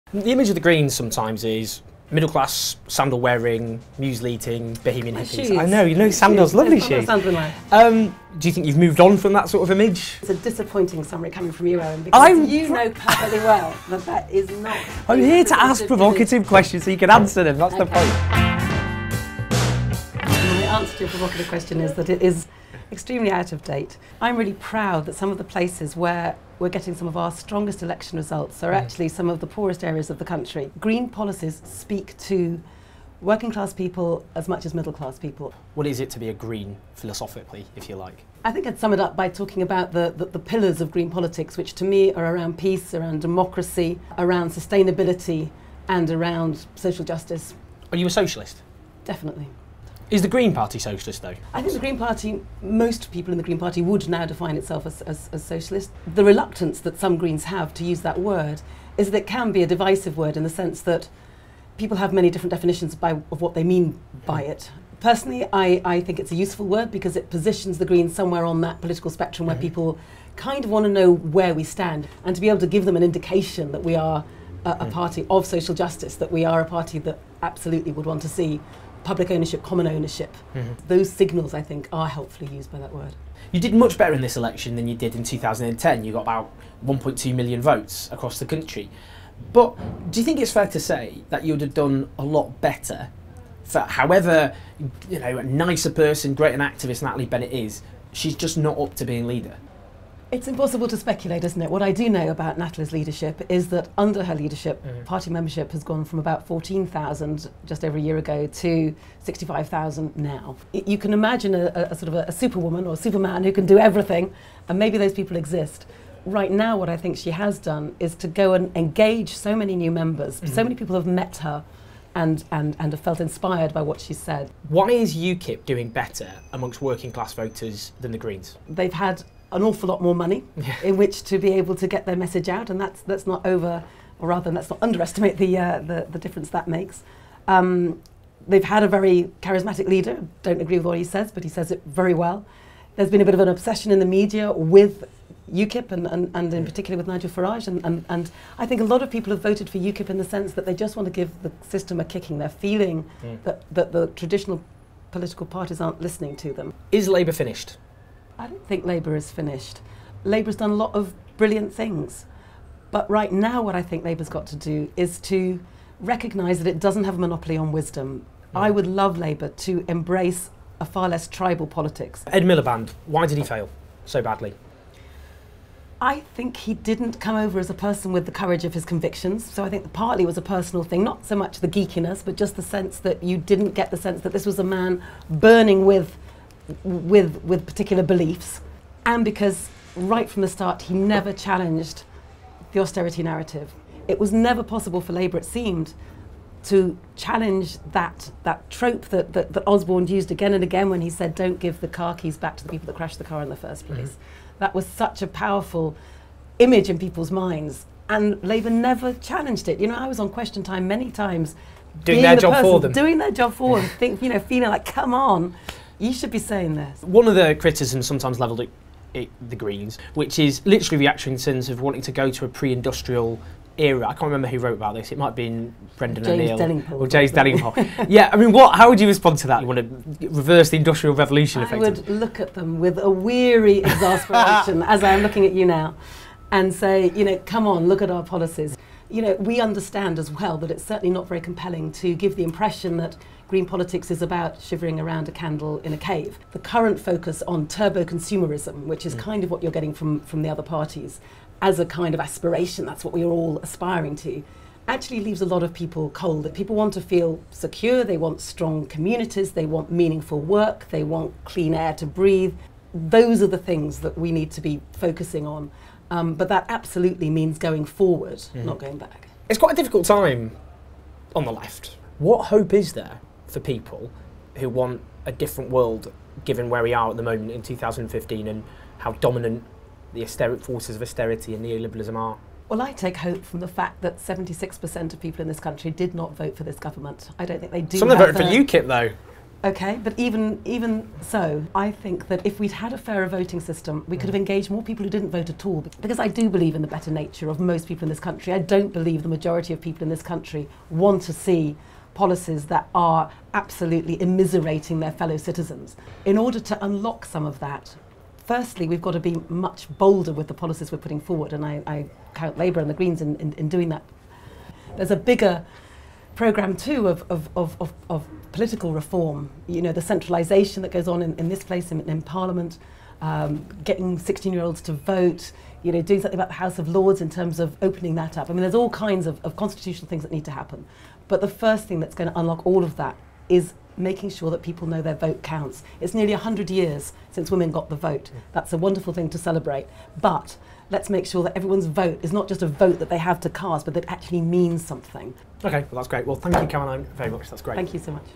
Owen Jones meets Caroline Lucas | 'Labour never challenged the austerity narrative'
I went to meet Caroline Lucas, the Green Party's only MP and former leader, to talk about what it means to be a Green, whether or not she's a socialist and to find out what hope she holds for the future.
We also chatted about why the Labour Party and Ed Miliband failed so badly at the last election, the need for a progressive coalition and what she thinks of the image of the Greens as a party for middle-class, sandal-wearing hippies.